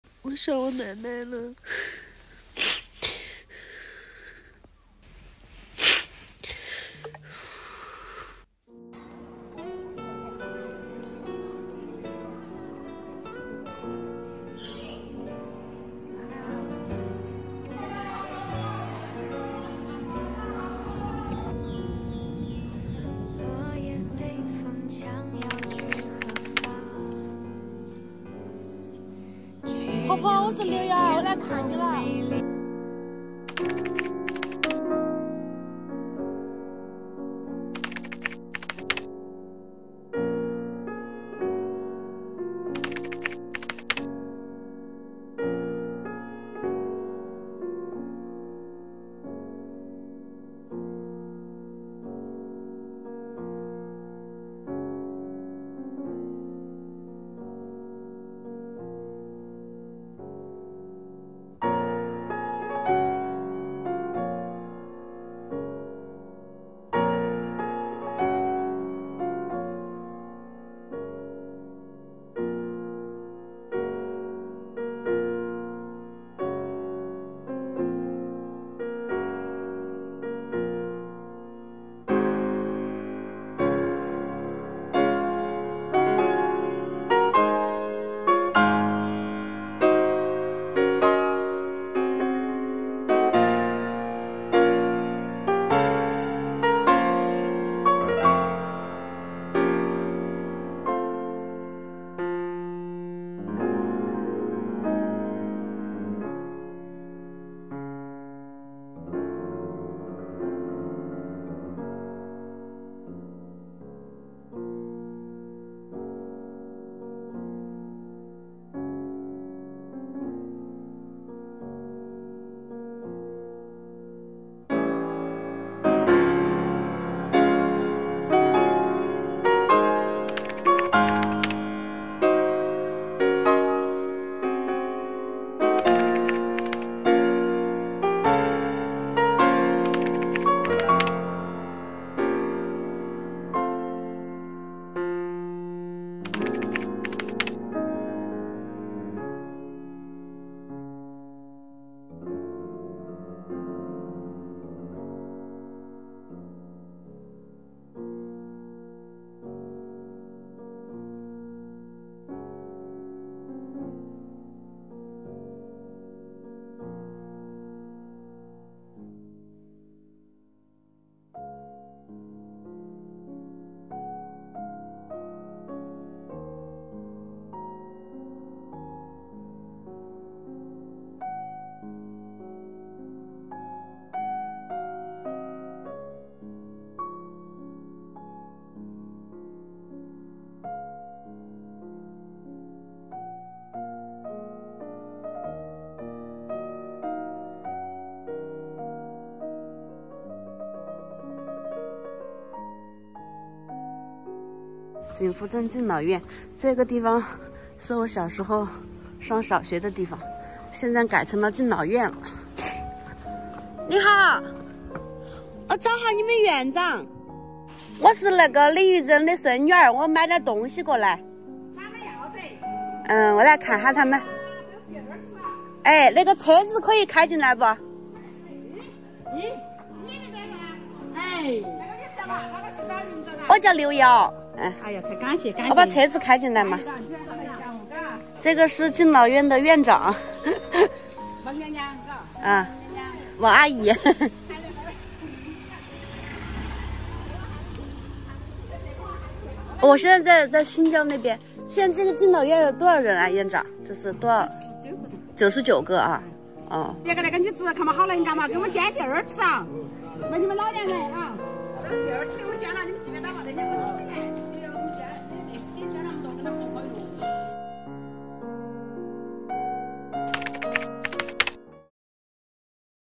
旅行按下暂停键，偷偷坐飞机回四川，想念奶奶哭成了泪人
记录和分享旅途中的美好